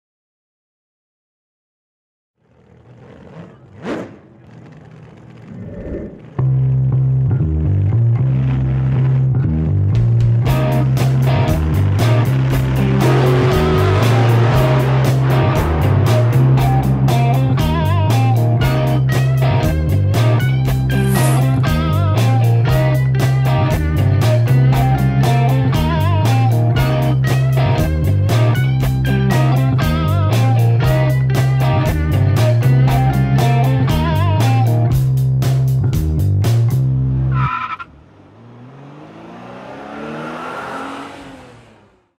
Swinger Style
Check out this trailer for Swinger Style, part of the Hot Rods series for sexy mechanics and fast cars! Available in ebook, print and audiobook.